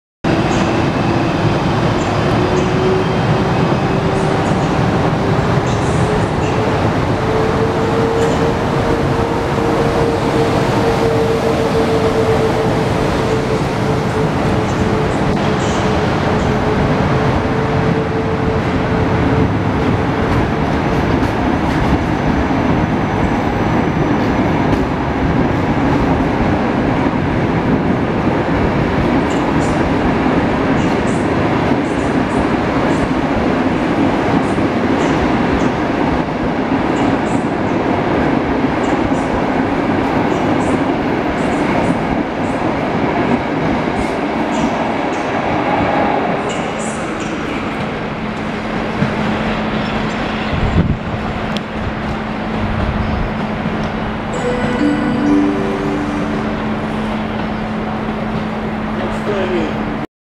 EP09-014 (EU: 150 003-8) z TLK 6507 "PiAST" (WR.GŁ.-GDY.GŁ.) - start z WR.GŁ.
EP09-014 (EU: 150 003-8) z TLK 6507 "PiAST" (WROCŁAW GŁÓWNY - GDYNiA GŁÓWNA)
startuje z toru 2, przy peronie 1 na stacji WROCŁAW GŁÓWNY.

Adres: ul.Piłsudskiego 105, 50-085 Wrocław
(osiedle: "Przedmieście Oławskie" na dzielnicy: "Stare Miasto"),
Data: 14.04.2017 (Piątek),
Godzina: 16:29.